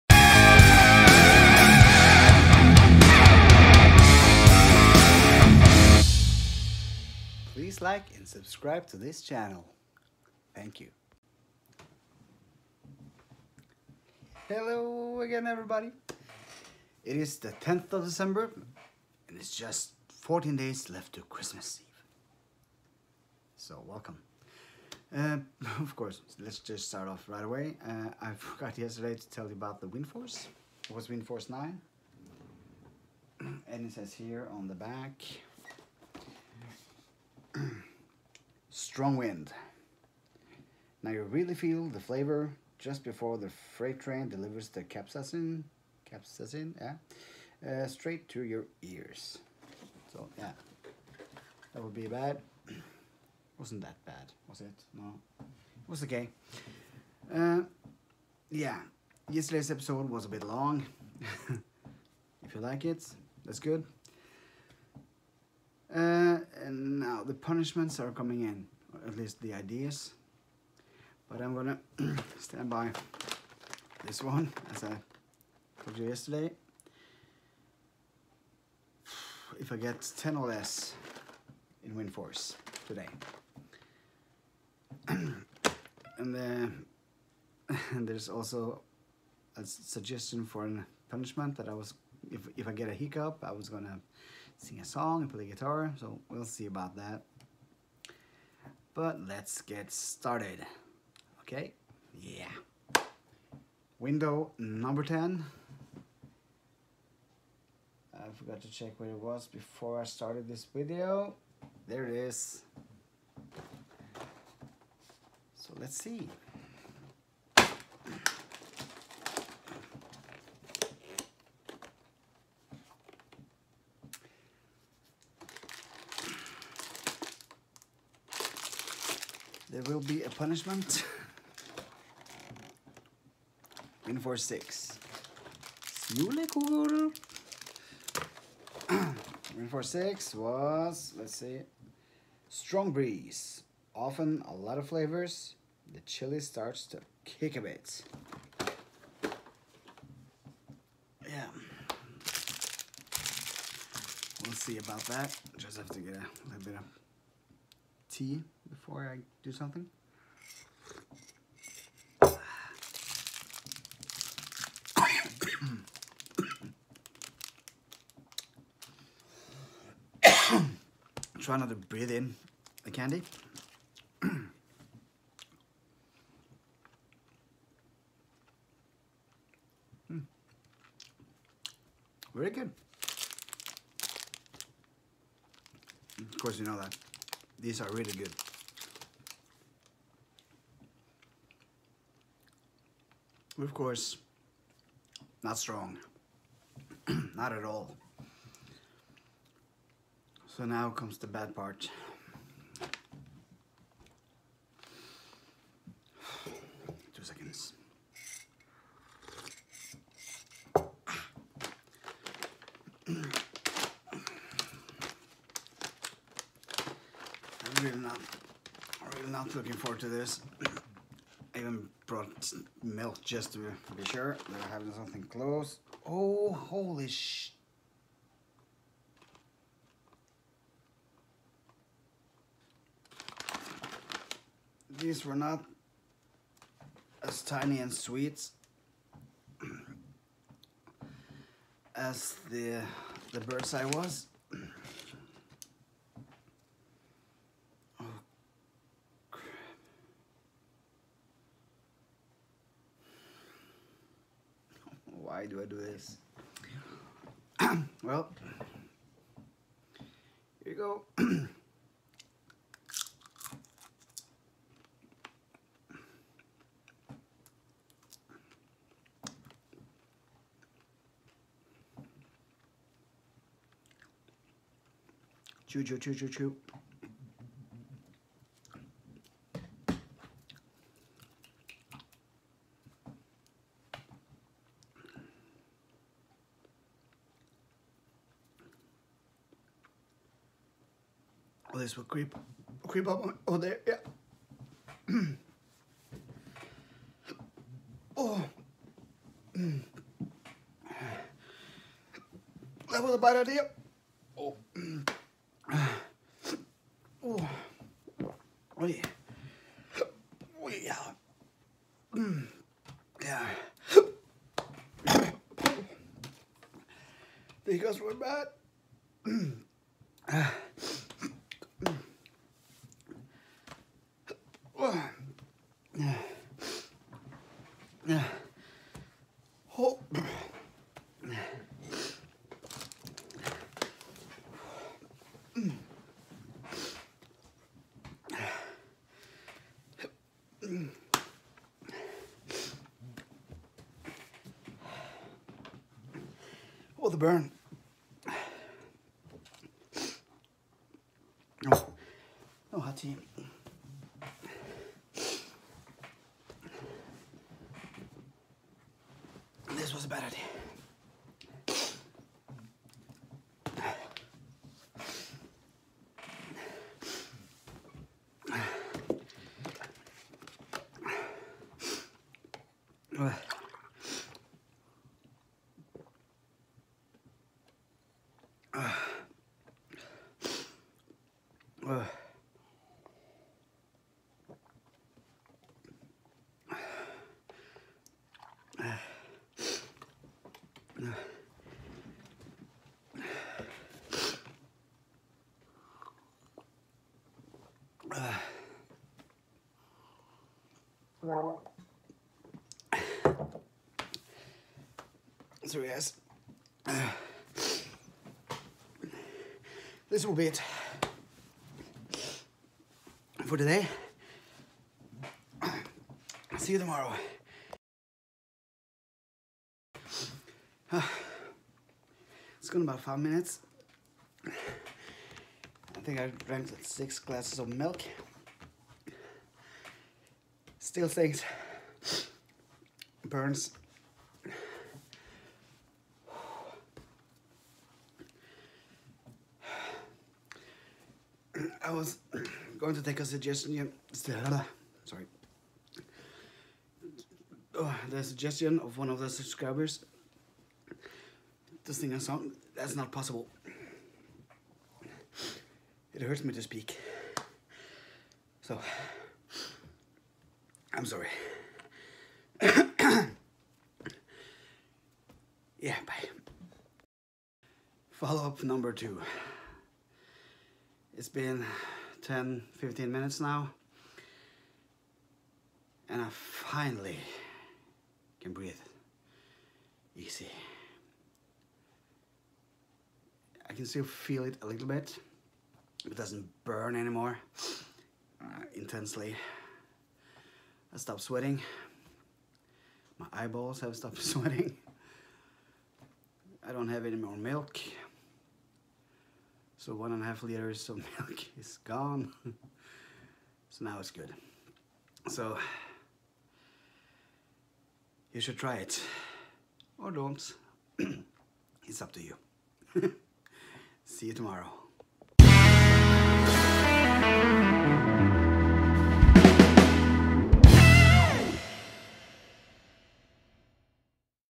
Episode 10 Chili Klaus Jingle Balls Christmas Calendar 2019
Luke 10 av Chili Klaus sin julekalender. Opening window nr 10 in Chili Klaus`s christmas calendar Jingle Balls